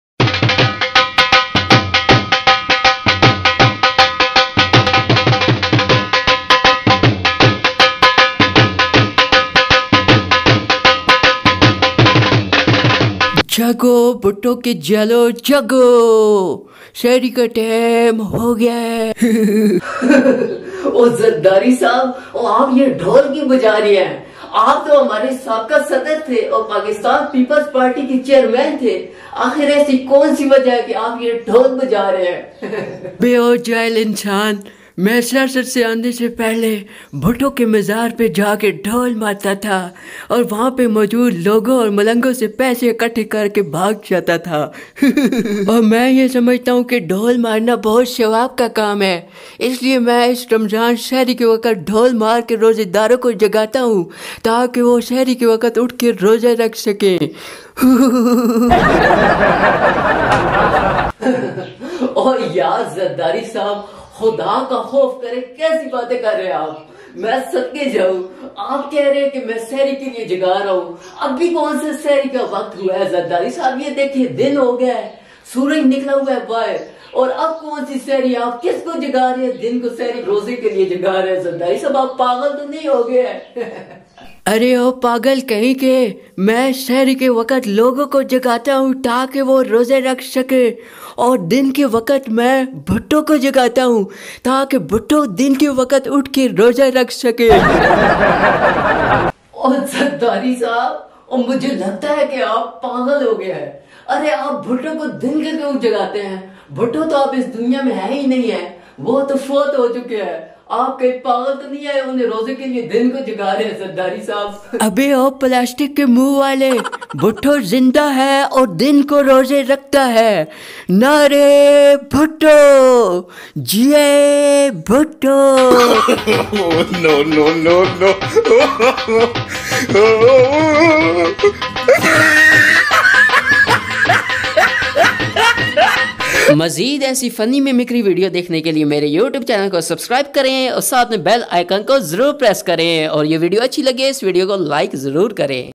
Zardari Dhol Wala Funny Video
Asif Ali Zardari Dhol Wala Funny Mimicry. Ppp Chairman.